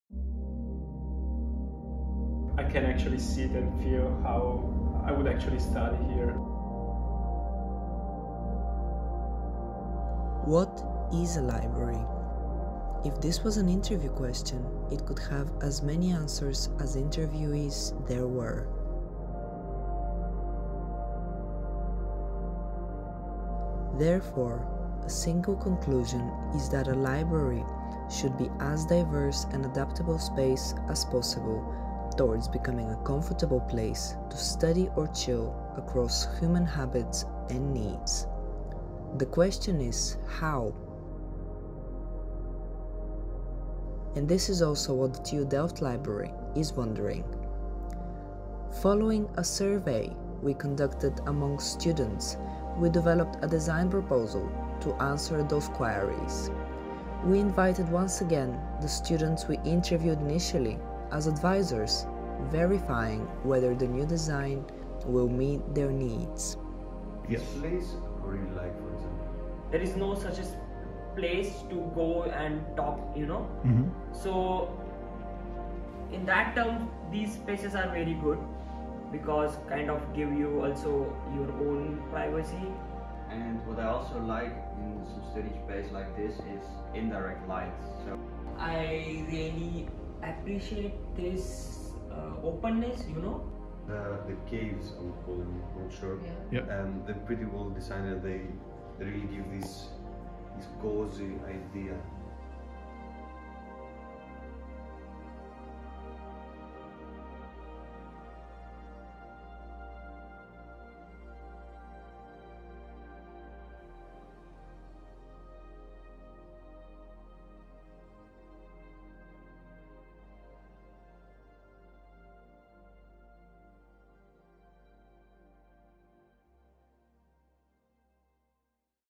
"Apart But Together" - Interactive Architecture Studio MSC2 2022 - TU Delft RB Lab
The studio Interactive Architecture (IA) focuses on the impact of robotics and Artificial Intelligence (AI) on architecture and building construction. The studio operates at the scale of architectural inserts situated within the urban context with the aim to address global challenges.

The project "Apart But Together" is based on the integrity of AI-supported and data-driven Design-to-RoboticProduction and Operation (D2RP&O) processes that link computational design to robotic materialization.

The project targets to implement a globally applicable system that purposefully entangles functional and spatial typologies in order to improve the user experience inside the TU Delft Library in the senses of wayfinding, zonal distribution, and interactivity. All in all to allow people to be individualistic in a unified common area.

The development was supported by computer vision and deep learning methods to automate the recognition of architectural elements, object tracking, and detection.